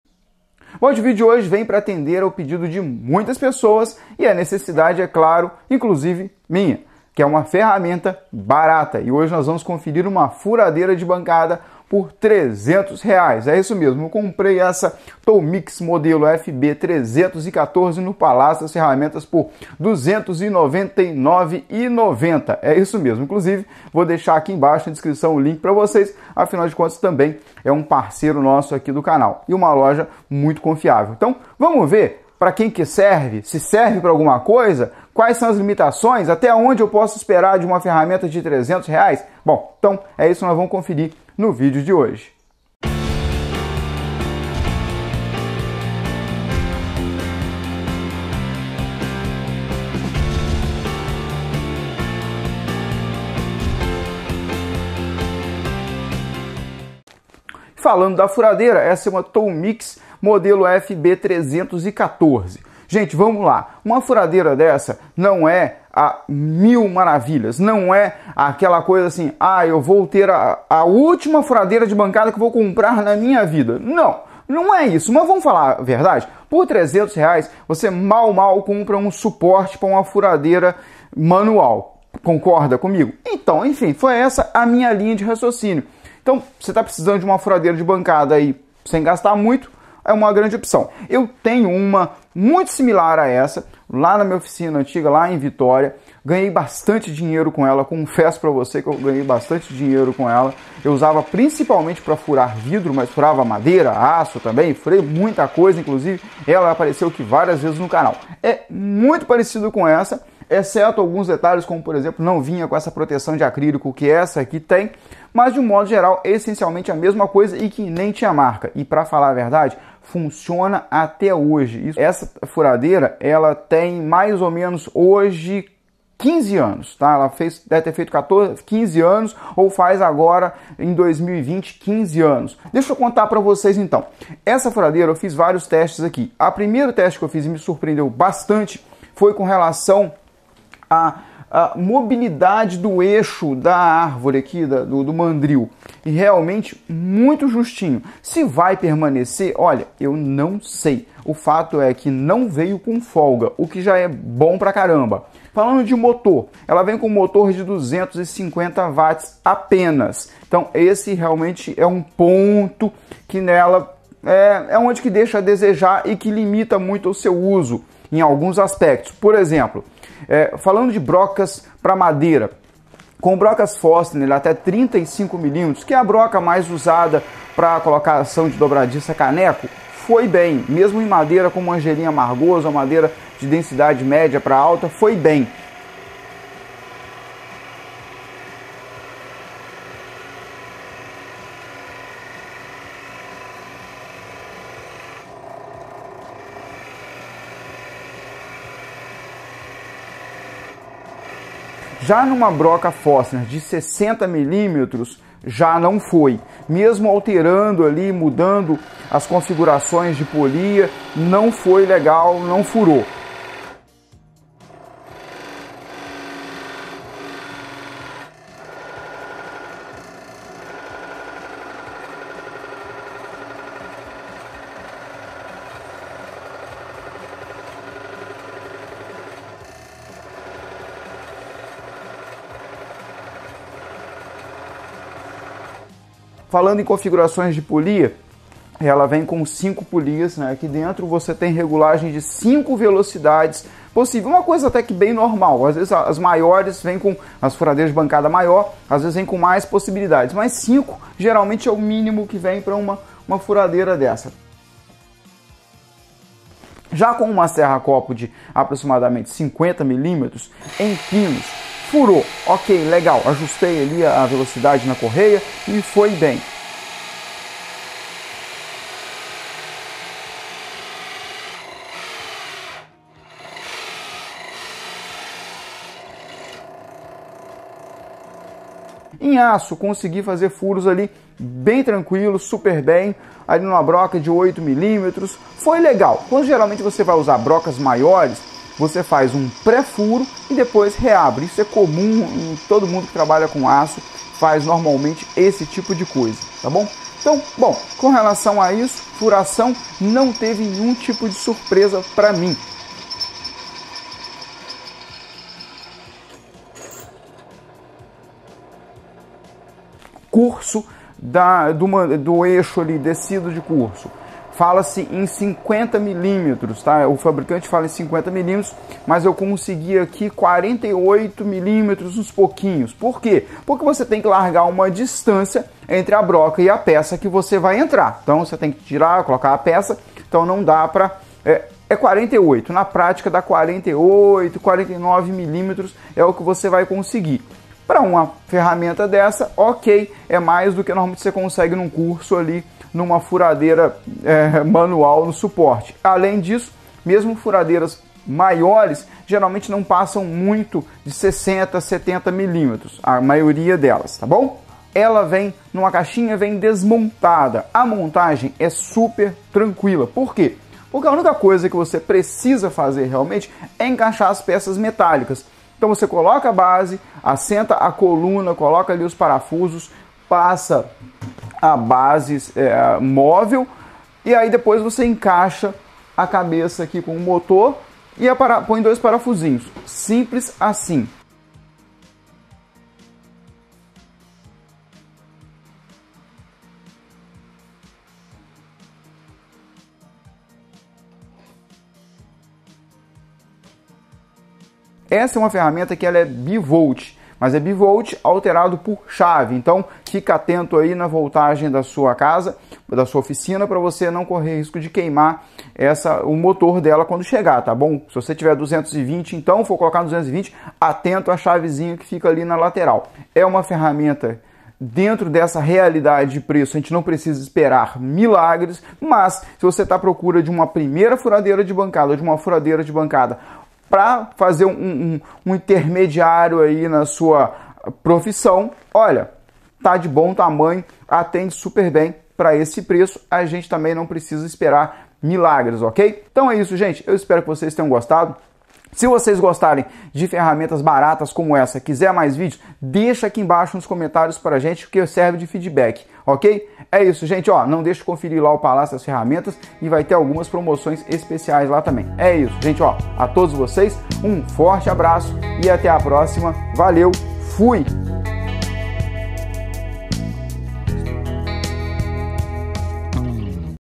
Comprei uma Furadeira de bancada por RS299,90. Será que Presta?
Análise de uma furadeira de bancada baratinha que comprei, confira os testes e veja se ela atende suas necessidades na oficina,

Onde mais me encontrar
APOIO
A ferramenta que você precisa. Quando precisa.

Visite um Redecenter, a rede de lojas da Redelease, espalhadas por todo o Brasil.